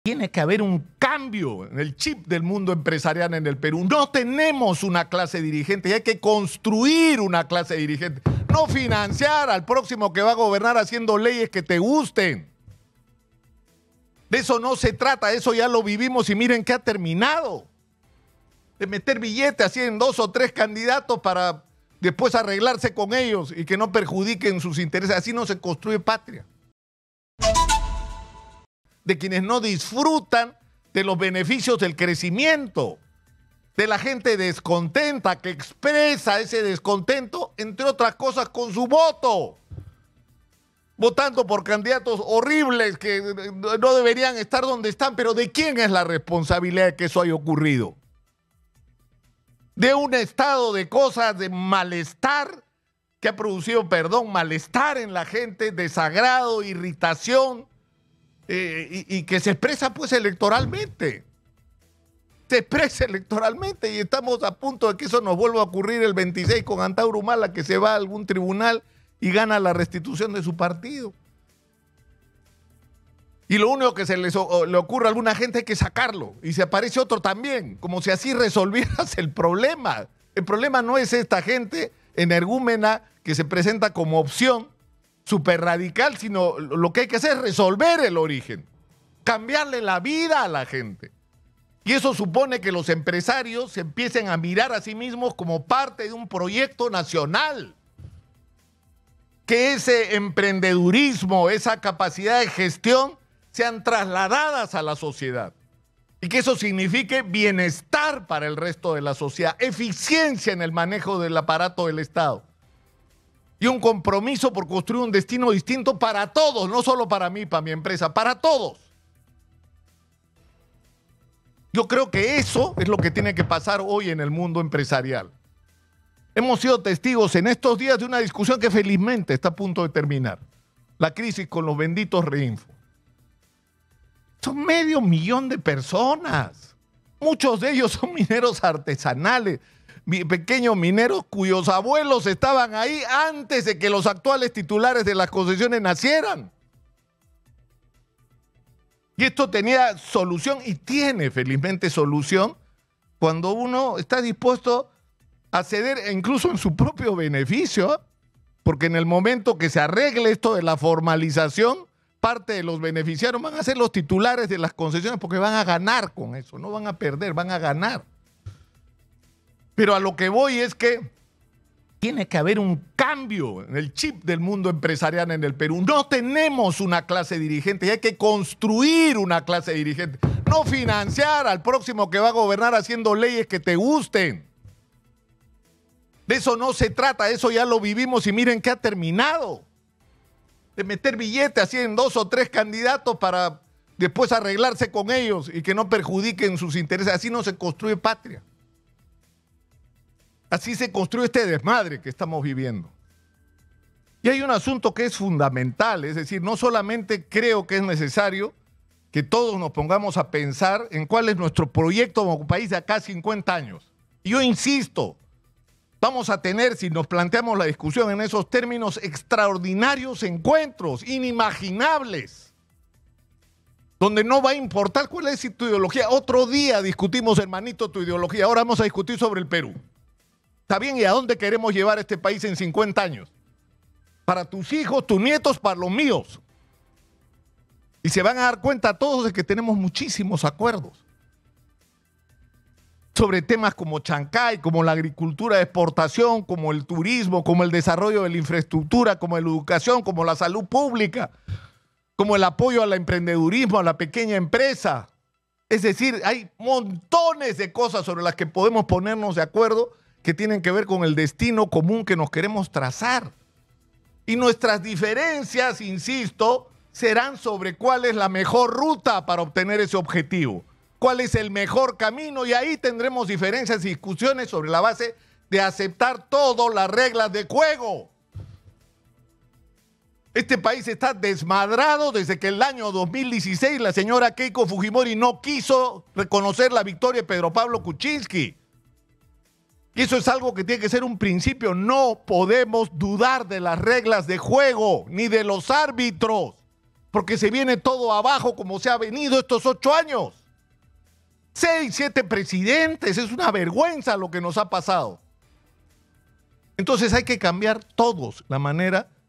🔴🔵Lúcar: Tiene que haber un cambio de chip en clase empresarial, no financiar al próximo gobernante
Nicolás Lúcar, conductor de Exitosa, sobre la clase empresarial: "Tiene que haber un cambio de chip, no tenemos una clase dirigente, hay que construirla, no financiar al próximo gobernante".

Exitosa Perú
"Exitosa: La voz que integra al Perú"

Corporación Universal